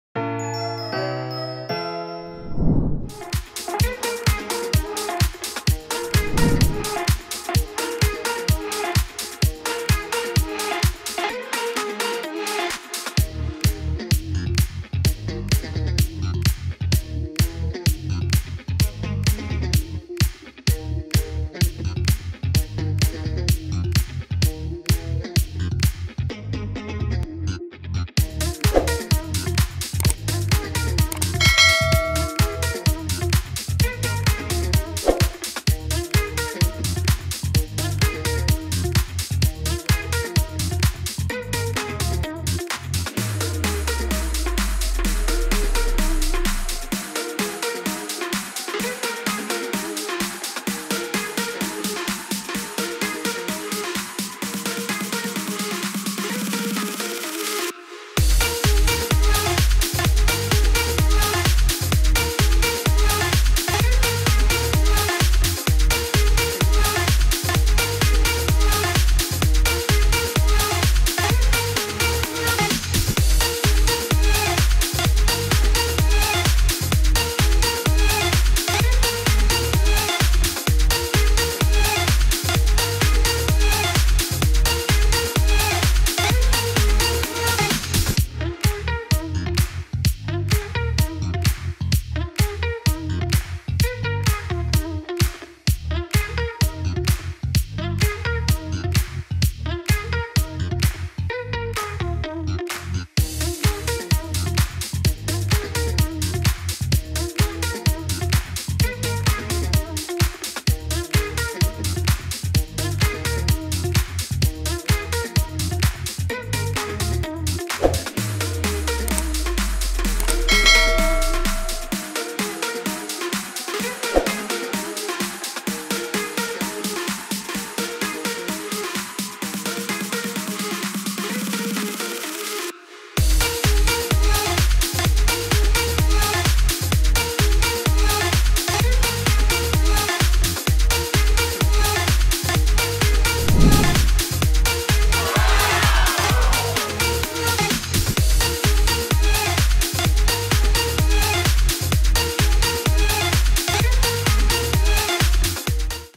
Oddbods coloring Pages How to Color Oddbod Zee coloring pages
Hi !  In this video I coloring  Oddbods  Zee  coloring pages !!